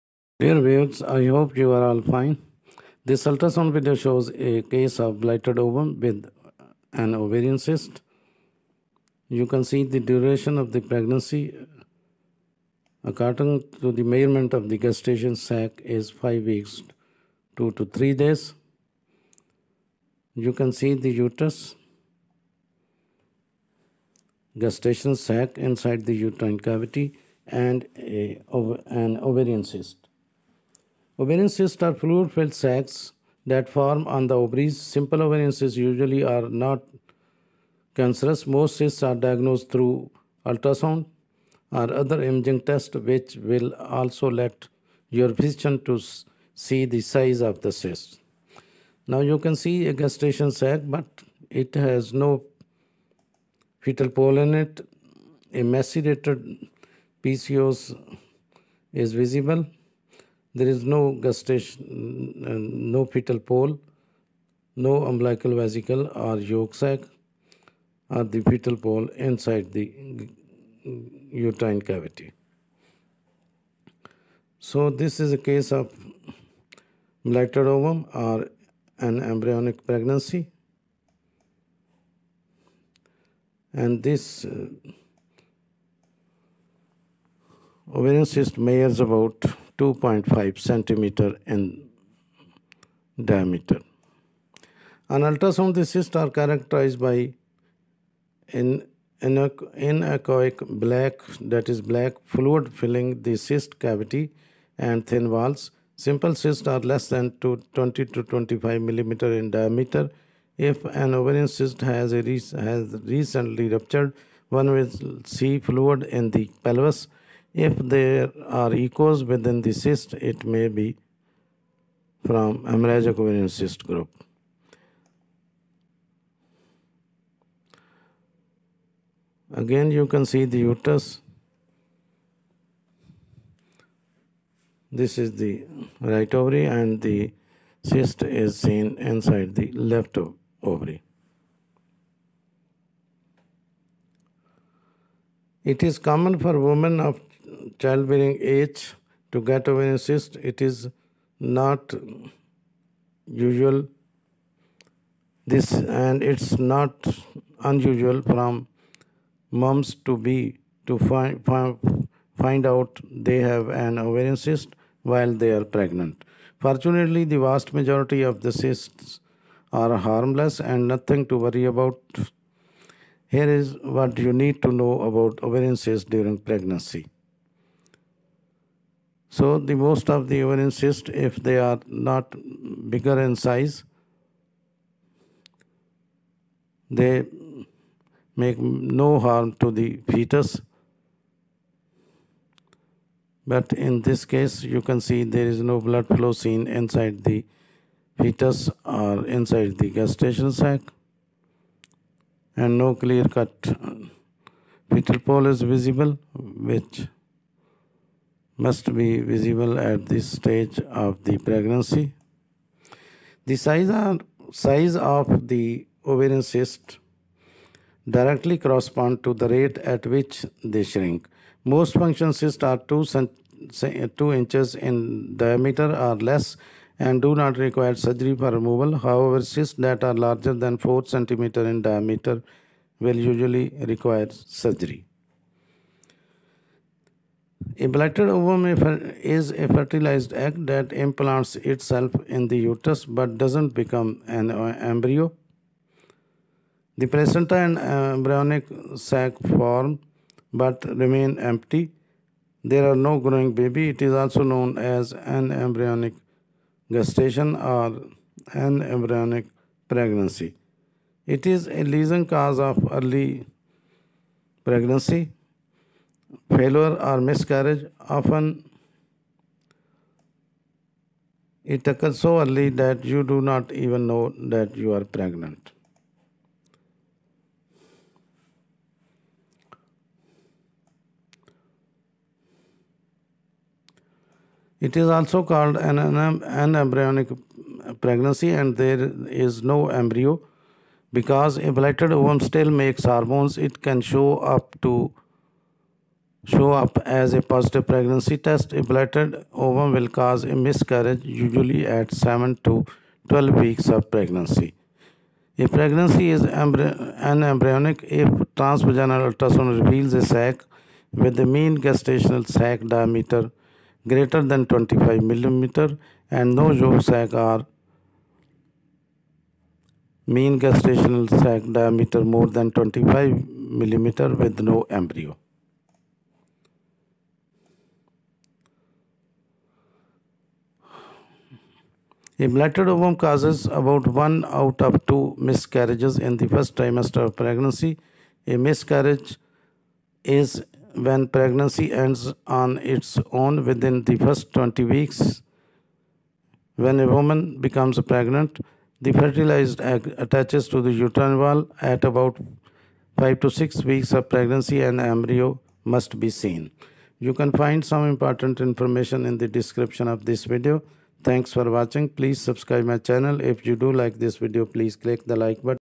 Blighted Ovum with an Ovarian Cyst.
This video shows Blighted Ovum with an Ovarian Cyst.
Ovarian cysts are fluid-filled sacs that form on the ovaries. Simple ovarian cysts usually are not cancerous. Most cysts are diagnosed through ultrasound or other imaging tests, which will also let your physician see the size of the cysts.
On ultrasound, the cysts are characterized by anechoic (black) fluid filling the cyst cavity and thin walls. Simple cysts are less than 20-25 mm in diameter. If an ovarian cyst has recently ruptured, one will see fluid in the pelvis. If there are echoes within the cyst, it may be from hemorrhage.
It's common for women of childbearing age to get ovarian cysts, and it's not unusual for moms-to-be to find out they have an ovarian cyst while they're pregnant. Fortunately, the vast majority of ovarian cysts are harmless and nothing to worry about. Here's what you need to know about ovarian cysts during pregnancy.
The size of a cyst directly corresponds to the rate at which they shrink. Most functional cysts are 2 inches in diameter or less and do not require surgery for removal. However, cysts that are larger than 4 centimeters in diameter will usually require surgery.
A blighted ovum is a fertilized egg that implants itself in the uterus but doesn't become an embryo. The placenta and embryonic sac form, but remain empty. There's no growing baby. It's also known as Anembryonic gestation or anembryonic pregnancy. It is a leading cause of early pregnancy failure or miscarriage. Often it occurs so early that you don't even know you are pregnant.
It is also called an 'anembryonic pregnancy' as there is no embryo (developing baby). Because a blighted ovum still makes hormones, it can show up as a positive pregnancy test. A blighted ovum will cause a miscarriage usually at 7 to 12 weeks of pregnancy.
A pregnancy is anembryonic if a transvaginal ultrasound reveals a sac with a mean gestational sac diameter (MGD) greater than 25 mm and no yolk sac, or an MGD more than 25 mm with no embryo.
A blighted ovum causes about one out of two miscarriages in the first trimester of pregnancy. A miscarriage is when a pregnancy ends on its own within the first 20 weeks. When a woman becomes pregnant, the fertilized egg attaches to the uterine wall. At about five to six weeks of pregnancy, an embryo should be present.
A blighted ovum, also called an anembryonic pregnancy, occurs when an early embryo never develops or stops developing, is reabsorbed, and leaves an empty gestational sac. The reason this occurs is often unknown, but it may be due to chromosomal abnormalities in the fertilized egg.
The gestational sac is typically visible on a transvaginal ultrasound somewhere between 3 to 5 weeks of pregnancy, or by the time the hCG has reached 1500 to 2000. Before that, even in a viable pregnancy, there is not going to be a visible gestational sac on an ultrasound.
Doctors can diagnose a blighted ovum using an ultrasound starting at around week 7 of pregnancy. Imaging will show a smaller than normal and empty gestational sac, which contains no embryo.
Because a blighted ovum still makes hormones, it can show up as a positive pregnancy test. A blighted ovum will cause a miscarriage usually at 7 to 12 weeks of pregnancy. Your body realizes the pregnancy is not developing properly and starts to shed blood and tissue from the uterus.
The gestational sac is the large cavity of fluid surrounding the embryo. Early embryogenesis consists of the extraembryonic coelom, also called the chorionic cavity. The gestational sac is normally contained within the uterus.
One of the more common types of miscarriages, known as an anembryonic pregnancy with an empty sac
In a blighted ovum, a gestational (embryo) sac forms and grows; however, the embryo does not develop. A blighted ovum is also known as anembryonic pregnancy. In the United Kingdom, miscarriage is the loss of an intrauterine gestational sac diameter (MGD) greater than 25 mm and no yolk sac.
The fetal pole is usually identified at 6.5 weeks with transabdominal ultrasound imaging and at 6 weeks with transvaginal ultrasound imaging, although it may not be seen until 9 weeks in some cases. When the fetal pole measures 7 mm or more, a fetal heartbeat should be detected.
The yolk sac is a membranous sac attached to an embryo, formed by cells of the hypoblast adjacent to the embryonic disk. This is alternatively called the umbilical vesicle by the Terminologia Embryologica (TE), though the yolk sac is far more widely used. In humans, the yolk sac is important in the early embryonic blood supply.
When in a follow-up ultrasound there's still no sign of a fetal pole (or of a gestational sac, which appears as a white rim around a clear center and will eventually contain amniotic fluid and enclose the developing baby) it means a miscarriage has occurred.